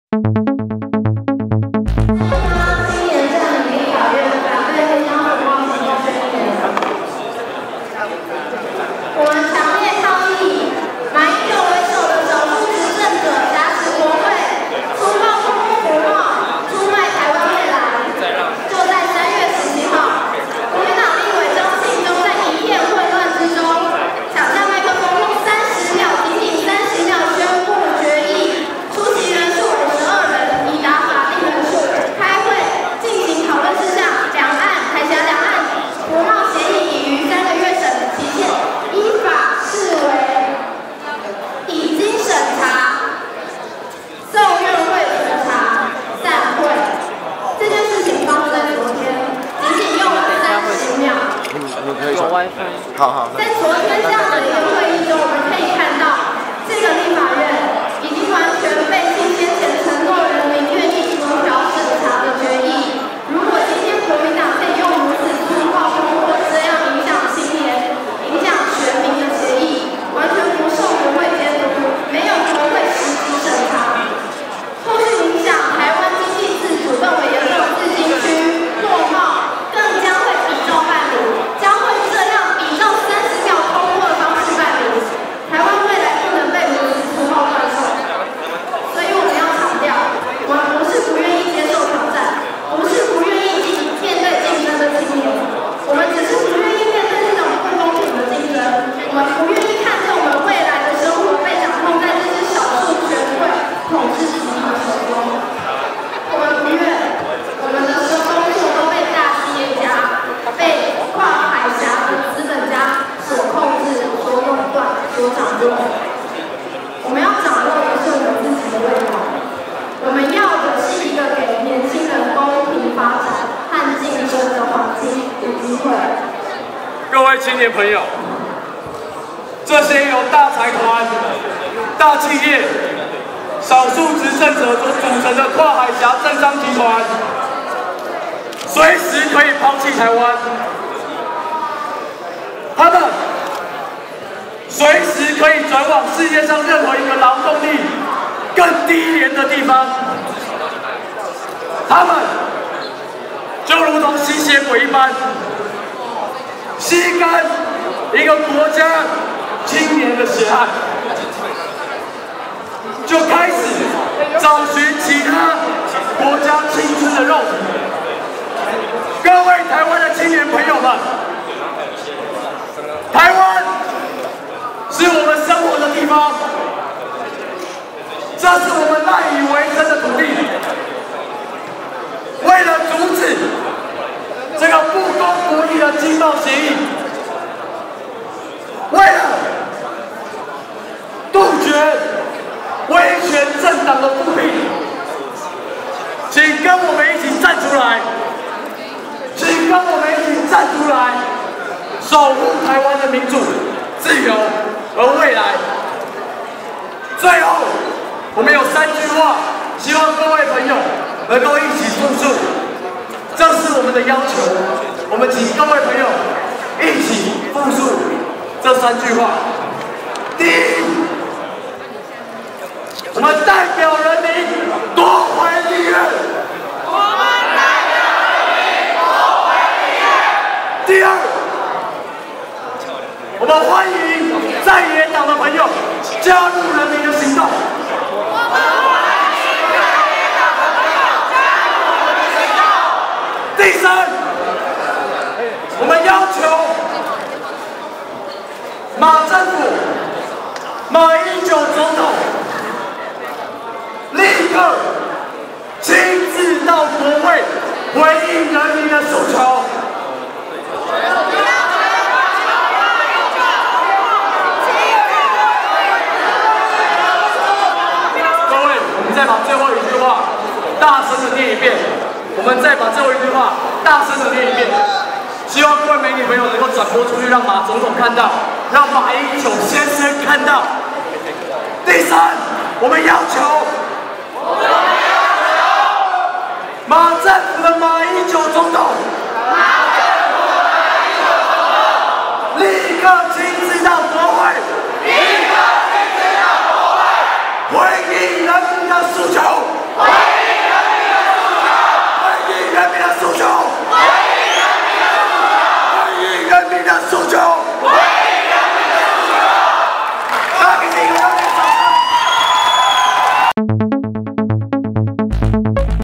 【沃草影音】 青年佔領立法院 反對黑箱服貿宣言
【沃草影音播映】青年佔領立法院 反對黑箱服貿宣言，民眾佔領立院議場稍早實錄！請大家持續關心！

轉述佔領立院青年的訴求：

1. 我們代表人民奪回立法院
2. 在野黨加入我們
3. 馬英九到立院回應民意